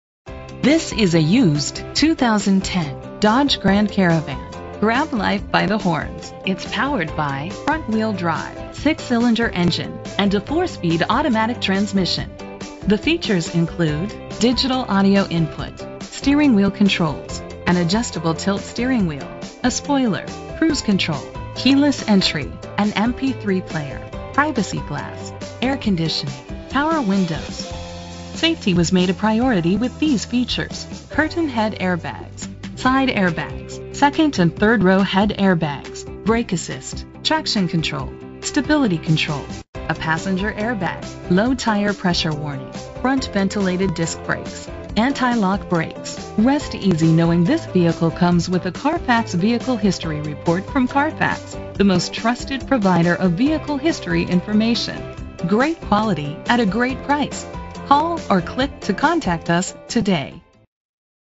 2010 Dodge Grand Caravan - Pensacola FL
Year: 2010
Make: Dodge
Model: Grand Caravan
Trim: Cargo
Engine: 3.3 liter 6 cylinder 12 valve
Transmission: 4-Speed Automatic
Color: Dark Titanium Metallic
Mileage: 62743
Address:
230 Beverly Parkway